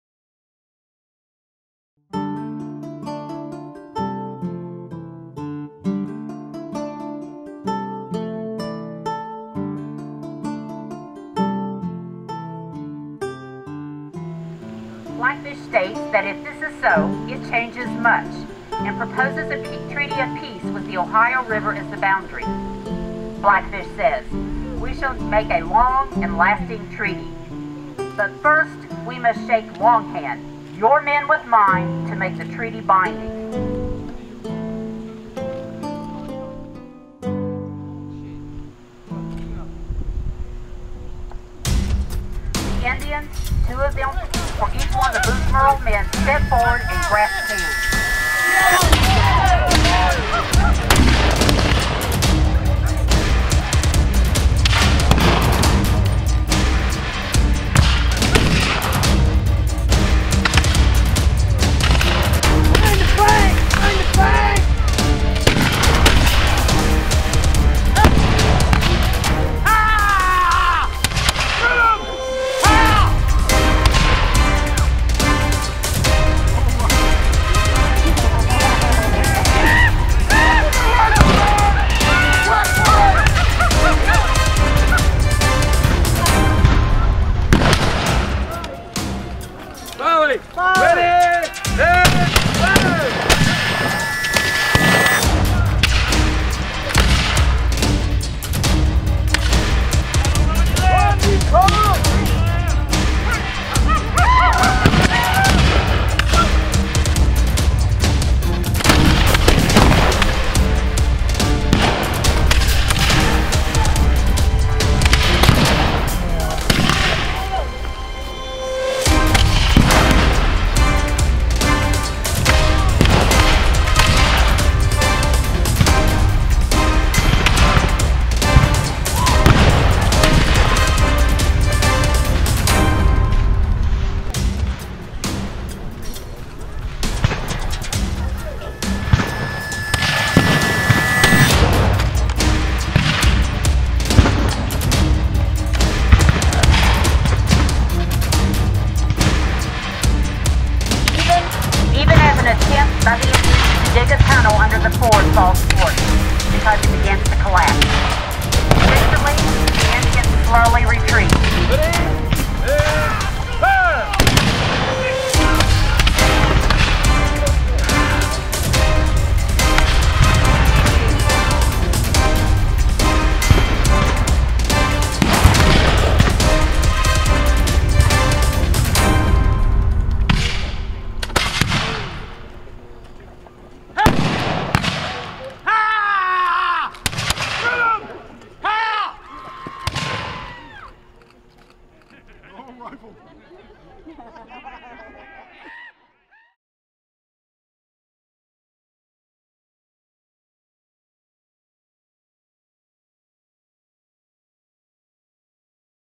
The Siege of 1778 at Fort Boonesborough
The Siege of Boonesborough took place in September 1778 during the American Revolutionary War. The attack on the Kentucky settlement of Boonesborough was led by Chief Blackfish, a Shawnee leader allied to the British. Months before the battle, Blackfish had captured and adopted Daniel Boone, the founder of Boonesborough. Boone escaped the Shawnees in time to lead the defense of the settlement. Blackfish's siege was unsuccessful and was lifted after ten days. Boone was then court-martialed by fellow officers who suspected him of having British sympathies. Boone was acquitted, but he soon moved away from Boonesborough.